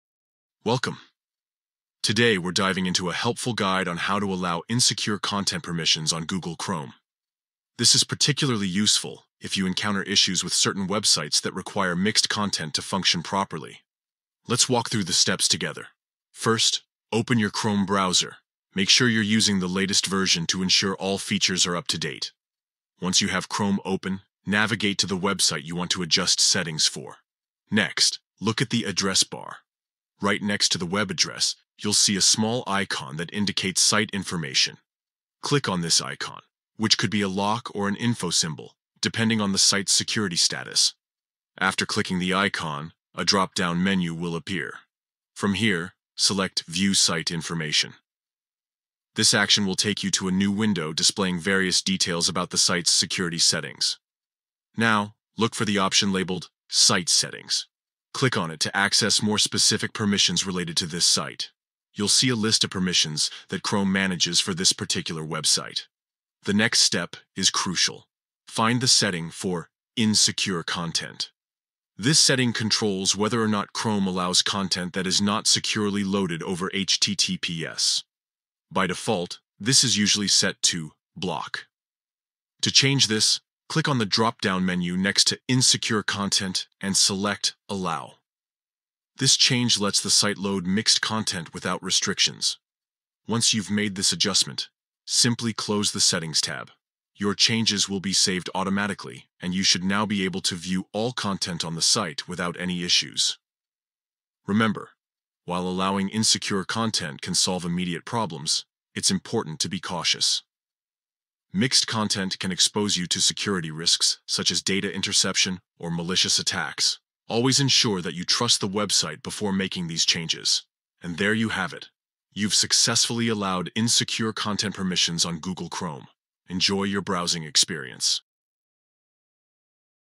How to Allow Site Insecure Content Permission On Google Chrome On PC (New Update)
Learn how to allow insecure content permissions on Google Chrome for specific sites with this easy step-by-step guide. This video will help you navigate through the new update and ensure your browsing experience is seamless.

1. Open Chrome Browser

2. Open the site you want

3. Next to the web address, click View site information

4. Open Site settings

5. Next to Insecure content, choose Allow

Chapters:
00:00 Intro
00:12 Soultion
01:05 Outro